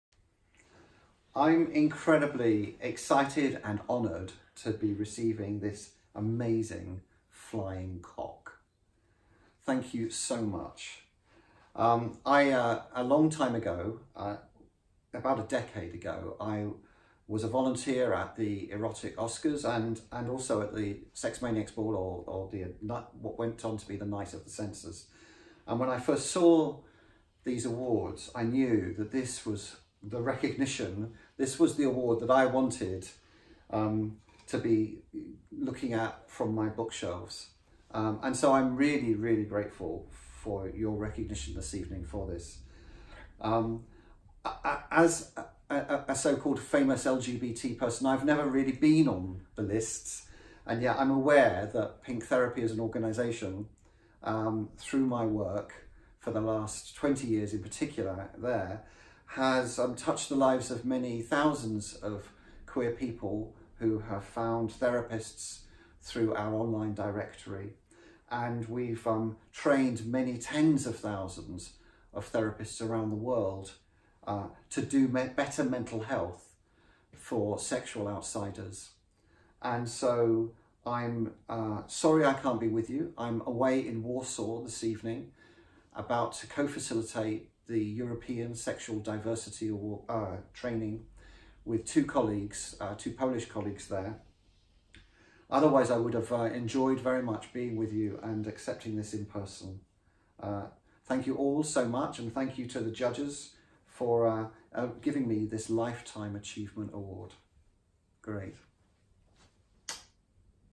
Lifetime Achievement Award
Tonight I was honoured to receive the Lifetime Achievement award from the Sexual Freedom Awards for long-standing dedication to sexual freedom, exploration and expression.
I was unable to attend the ceremony in person as I am away in Warsaw about to co-facilitate the European Sexual Diversity Training and so made this short acceptance speech.